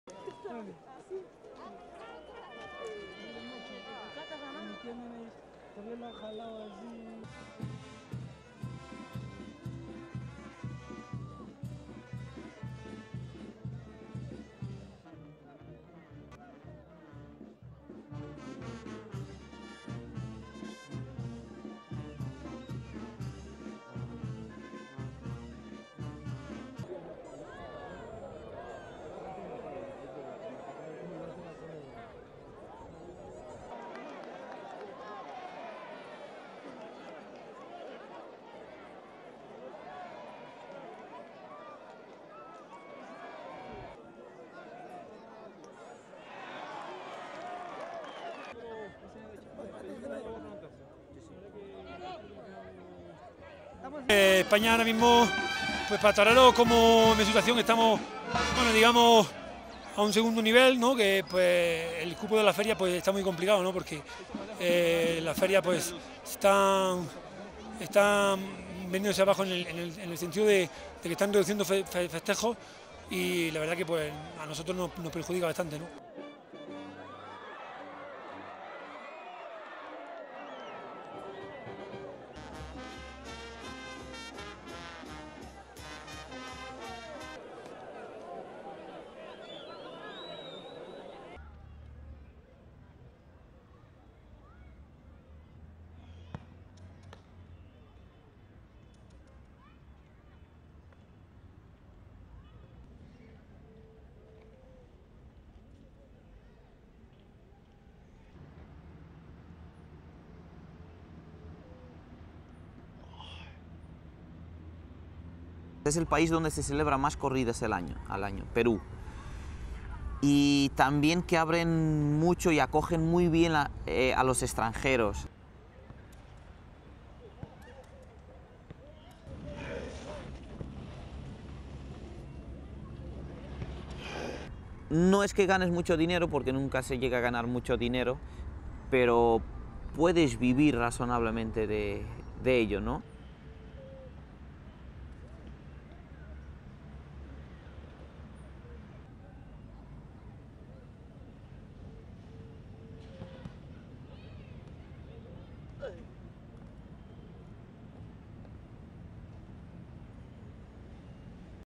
Economic crisis makes European bullfighters migrate to Peru
AP TELEVISION
Lachaqui - 22 July 2013
1. Wide of bullfighting ring, spectators sitting in the stands
2. Various of bullfighters making their entrance into ring
3. Wide top shot of bullfight
4. Wide of Spanish bullfighter, Francisco Torres Jerez during bullfight
5. Wide of Jerez stabbing bull with 'banderilla', a type of spear used in bullfighting
6. Jerez being interviewed
7. SOUNDBITE: (Spanish) Francisco Torres Jerez, Spanish bullfighter:
"In Spain right now, for a bullfighter, the situation is, we say, at a second level, the capacity of the festivals is very complicated because they are decreasing, in the sense that they are reducing the festivities and the truth is that this is harmful to us."
8. Wide of Jerez getting butted by bull
9. Wide of Jerez in ring
10 .Wide of people gathered on hillside looking down on bullfighting ring
Lima - 3 August 2013
11. Wide of Portuguese bullfighter, Nuno Casquinha, training in a park in Lima
12. Close of Casquinha's hand holding a red bullfighting cape as he practises in the park
13. Mid of Casquinha moving arm while holding a red bullfighting cape
14. SOUNDBITE (Spanish) Nuno Casquinha, Portuguese bullfighter:
"Peru is the country that hosts the most bullfights per year.  And they also welcome and embrace foreigners."
15. Wide of Casquinha practising with his bullfighting cape
16. Close of Casquinha's cape with his name written on one side
17. SOUNDBITE (Spanish) Nuno Casquinha, Portuguese bullfighter:
"It's not that you'll be earning a lot of money because you don't get to make a lot of money but you make a reasonable living from it."
18. Tilt from back of Casquinha's jersey with the letters "Portugal" to his hand holding a red bullfighting cape
19. Wide of Casquinha practising his hand and body movements in the park
STORYLINE
Dozens of European bullfighters, who have seen their dreams of practising their trade in the old world fade, are crossing the Atlantic to entertain remote towns in the Peruvian Andes.
The small towns may only have ramshackle bullfighting rings but the spirit of the sport is more alive than ever.
One of those who has made the move to the new world is Spanish bullfighter, Francisco Torres Jerez, who hails from the southern region of Almeria.
"In Spain right now, for a bullfighter, the situation is, we say, at a second level, the capacity of the festivals is very complicated because they are decreasing, in the sense that they are reducing the festivities and the truth is that this is harmful to us," he said.
Jerez has been contracted to perform in four bullfights in four rural villages in the Andes, one of which is in the village of Lachaqui, which sits nearly four thousand metres (13,123 feet) up in the mountains.
Decked out in traditional colourful and glittering attire, Jerez dazzles spectators at the simple bullfighting ring, eliciting gasps and cheers from the crowd.
The migration of the bullfighters has increased as a result of the economic crisis in Europe and changing tastes toward the traditional blood-sport.
Since 2008, 50 percent of bullfighting festivals have been cancelled across Spain, Portugal and the south of France.
Some 2,000 fights are still held yearly in Spain although crowd attendances have fallen drastically.
Portuguese bullfighter Nuno Casquinha is another of those who travelled to Peru.
He practises diligently in a park in Lima, the country's capital, moving his red cape gracefully and theatrically as he simulates a bullfight.
The pay, however, is modest.
Spain remains divided over bullfighting.